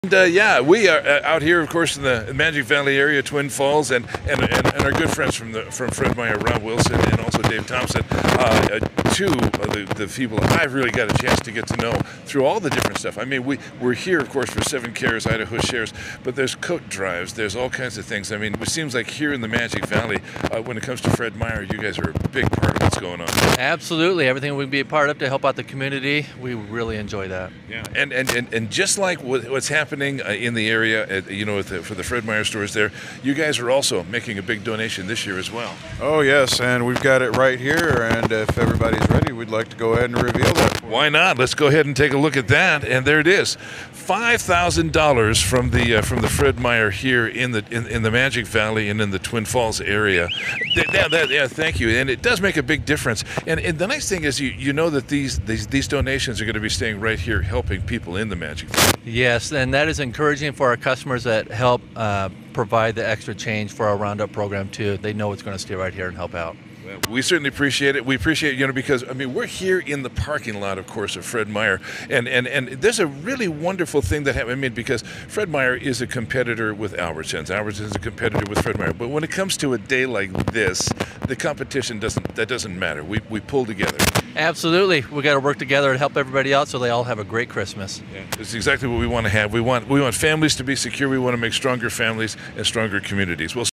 Twin Falls Fred Meyer gives $5,000 to help fight hunger in Magic Valley
Store managers presented a $5,000 check to the Idaho Foodbank, to help the hungry in the Magic Valley.